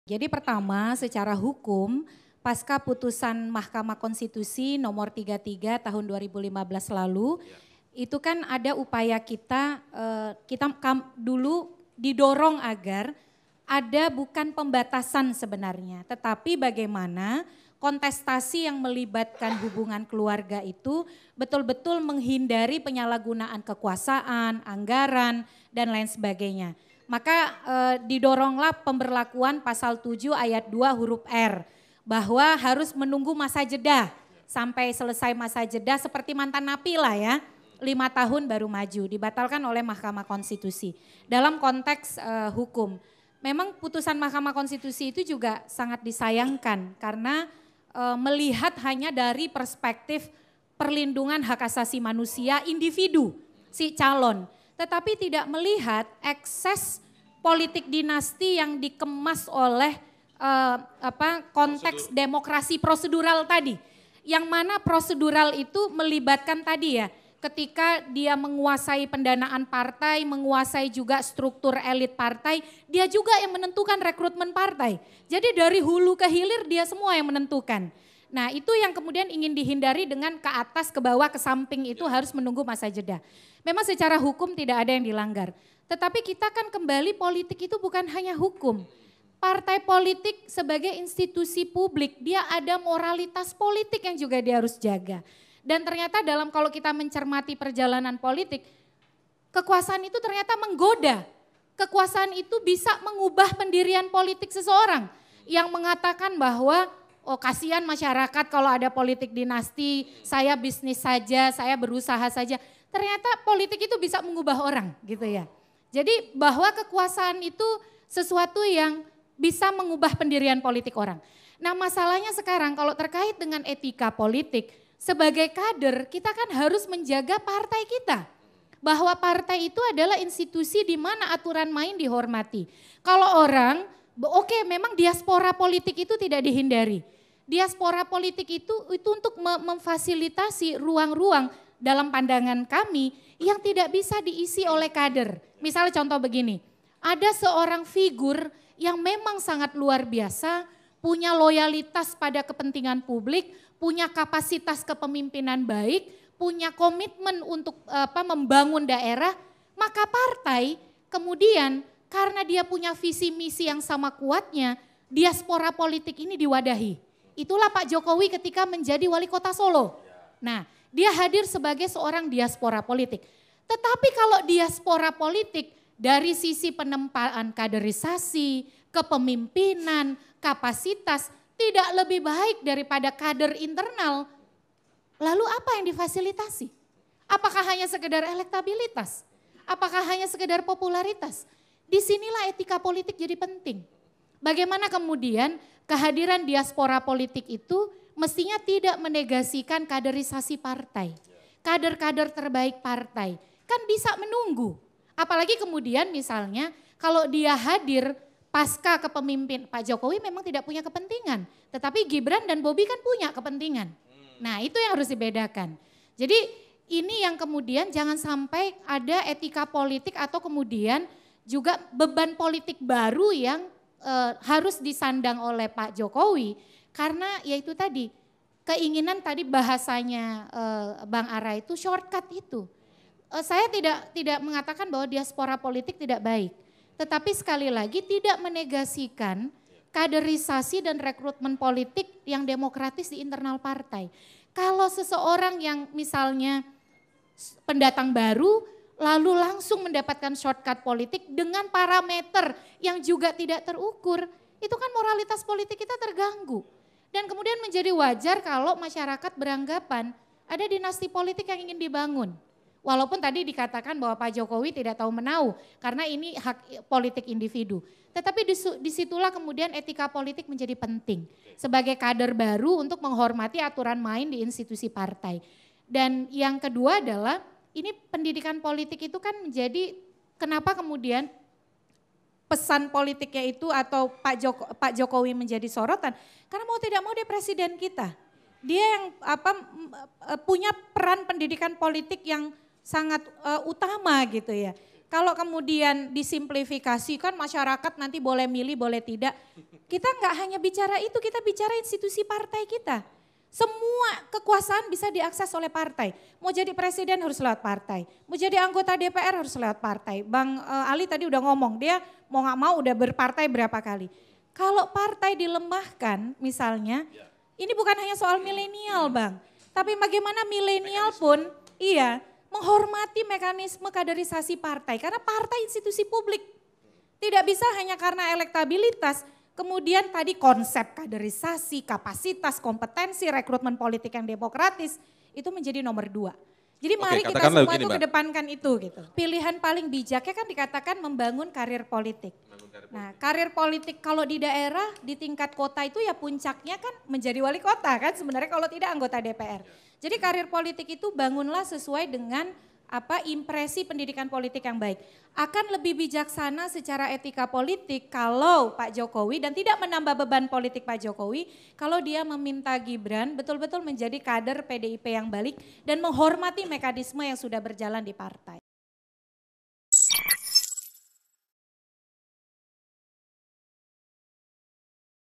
Perludem: Politik Dinasti Langgar Demokrasi
Direktur Eksekutif Perludem, Titi Anggraini mengatakan, moralitas politik akan terganggu jika ada seorang pendatang baru, lalu langsung mendapatkan shortcu politik dengan paramater yang tidak terukur.